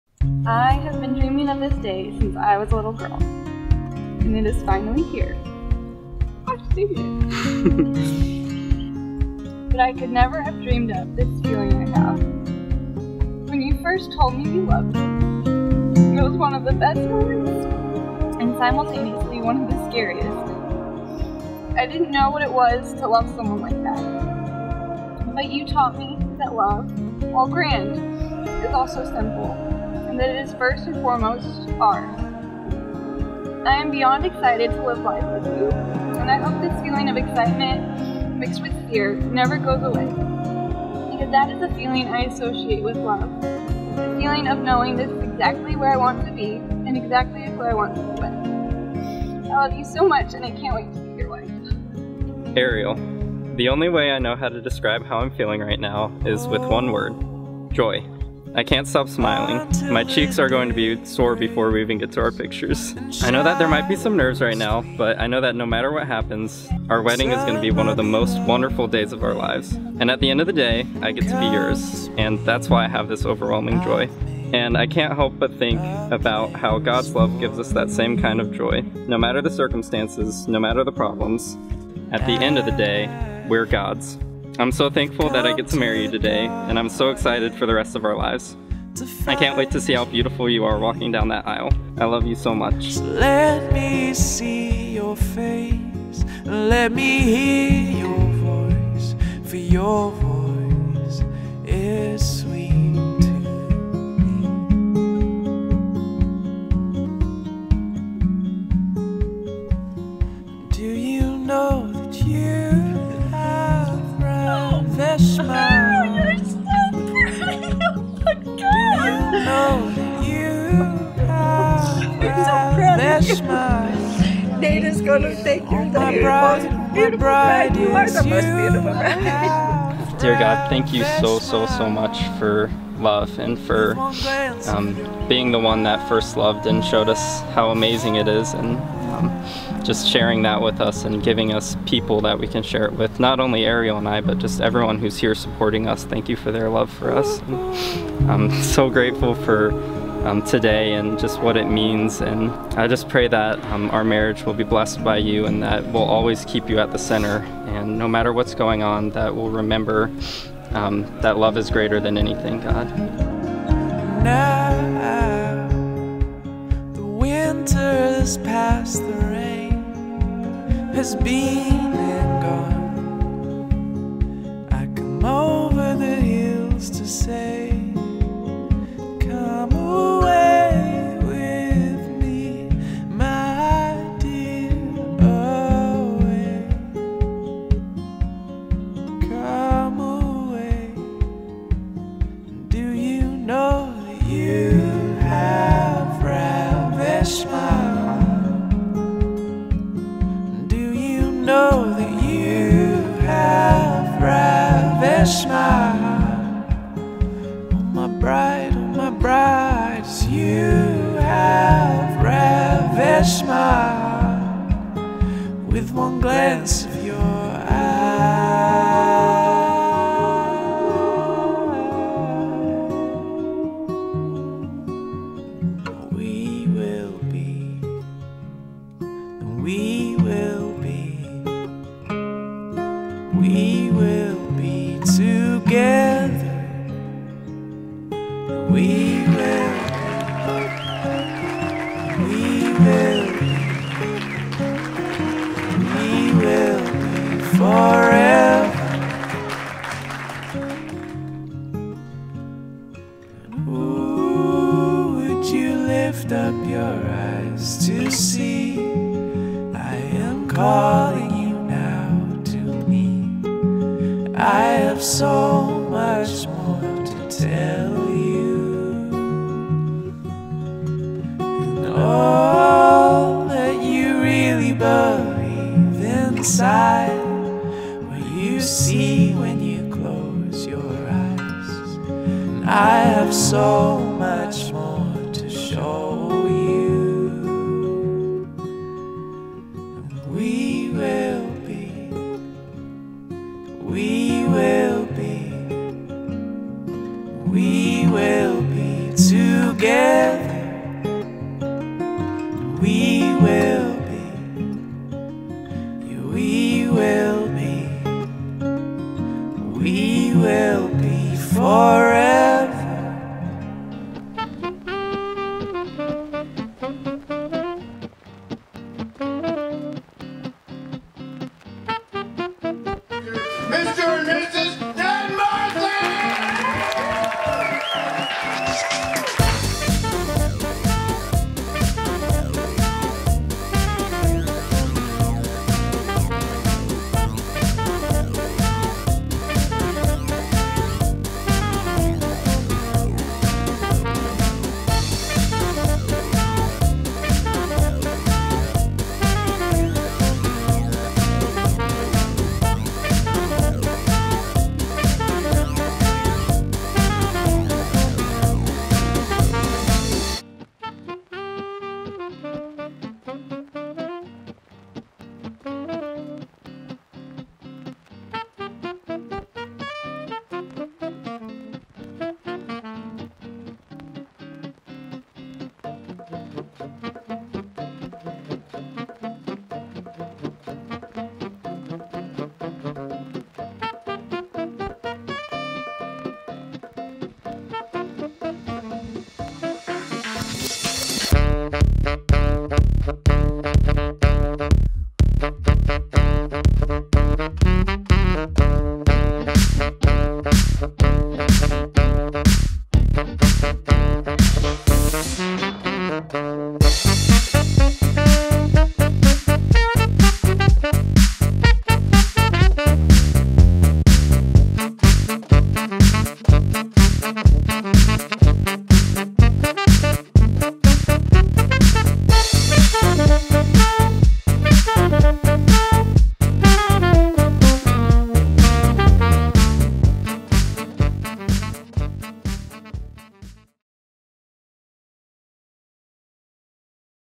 Becoming a Murphy
Our Wedding highlight

Videographer: Raelyn Lomison
Editors: Lionlight Films